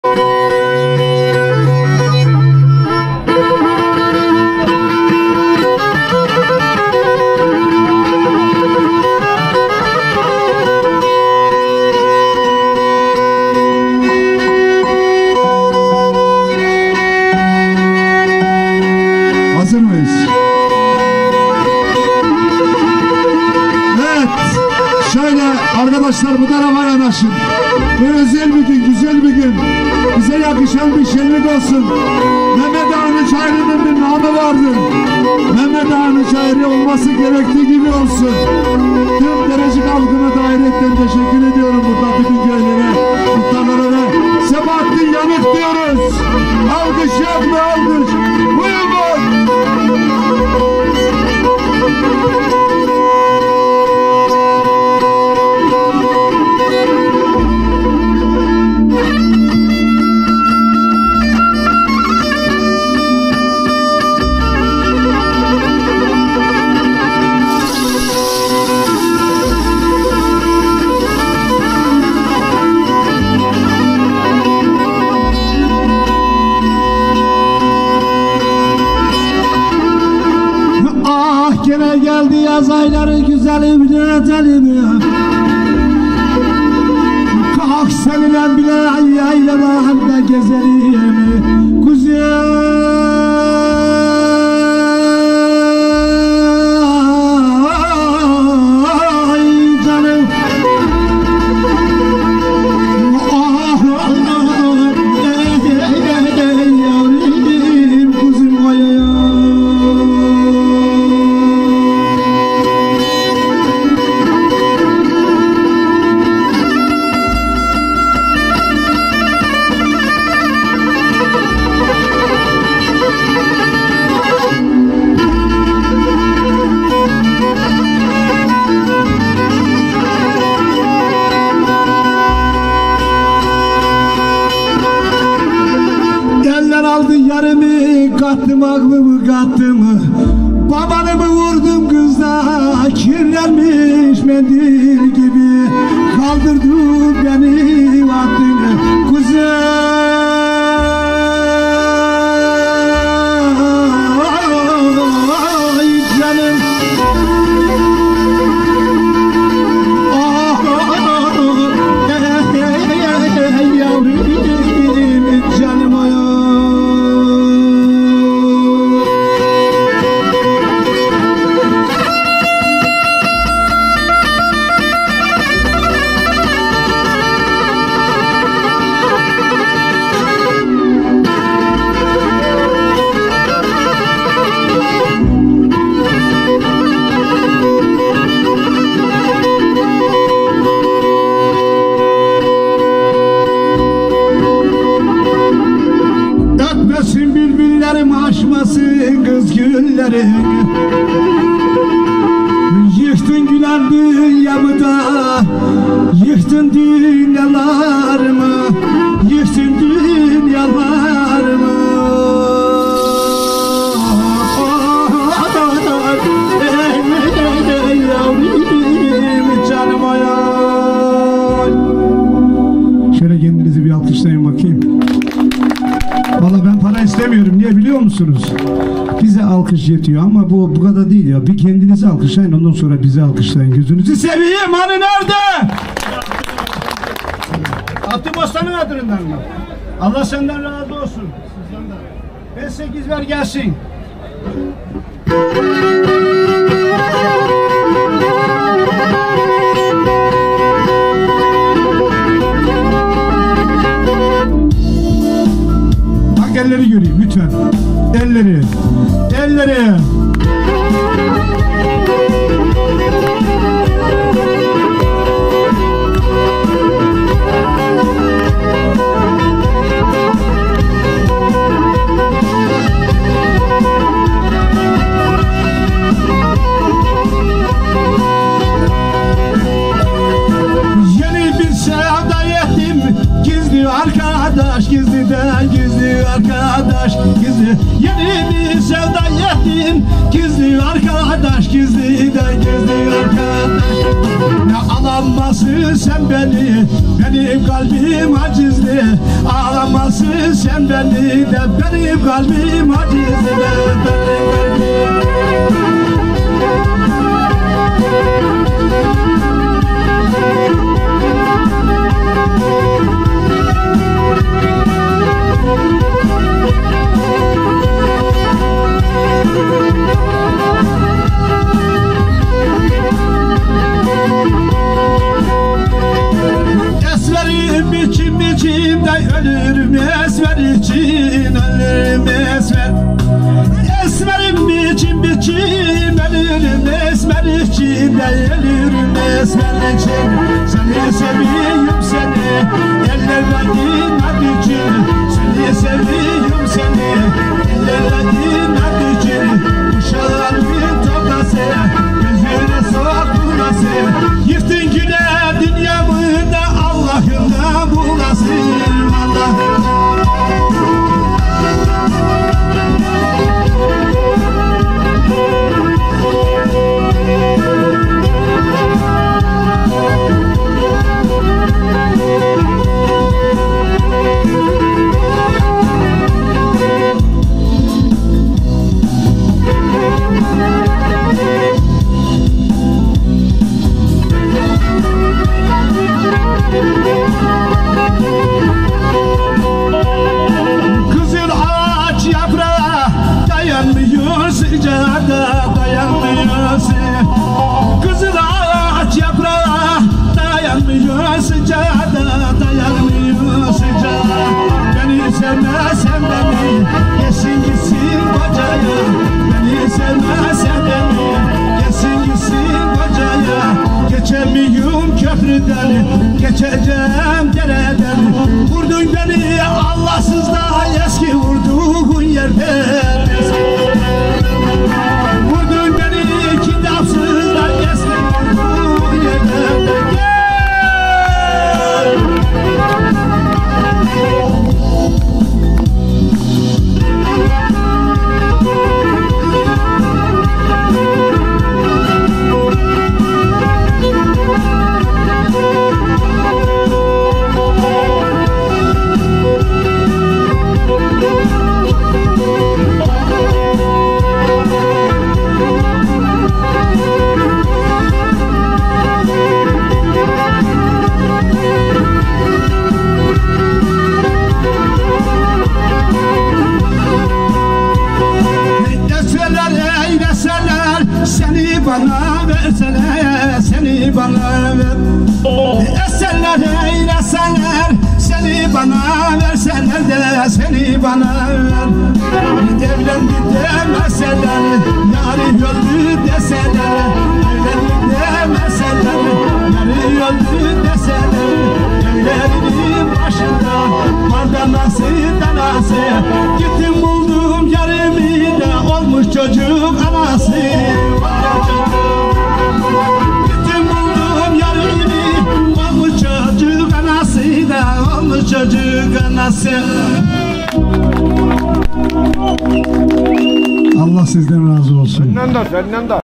Sebahattin Yanık ve Harika Yayla Havaları
Trabzon Akçaabat Acısu Mahallesinden selamlar. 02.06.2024 Mehmet Ağanın Çayırı Hıdırellez şenliklerinde Sebahattin Yanık abimizden harika yayla havaları. İyi seyirler.